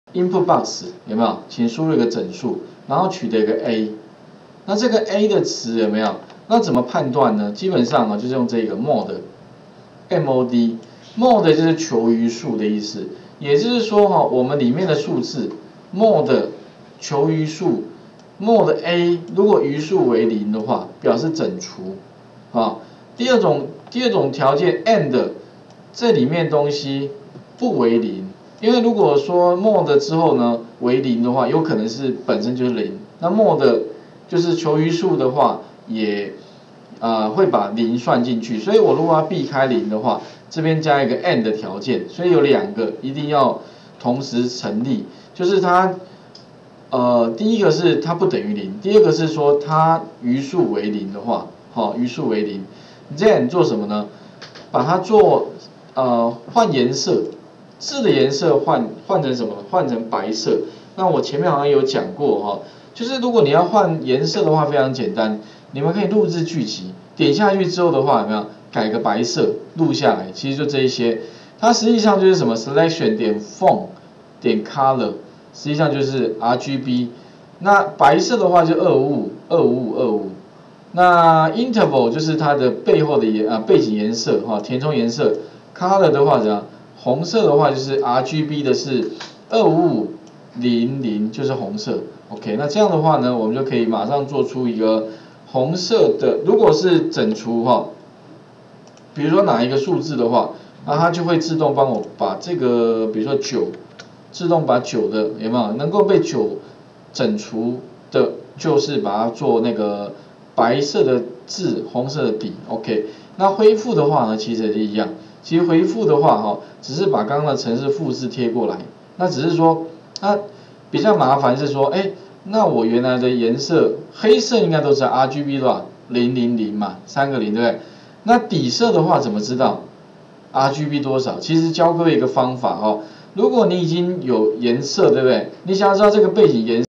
如何將某數改為白字紅底與回覆顏色EXCEL VBA與資料庫 吳老師
EXCEL函數與VBA懶人包

吳老師  102/12/2

課程特色：
1.如何將函數轉成VBA
2.VBA與資料庫快速結合

相關教學連結:
1.EXCEL VBA設計(自強基金會2012)第4次上課

2.如何把EXCEL"函數"變為 "VBA"？自強基金會2012第5次上課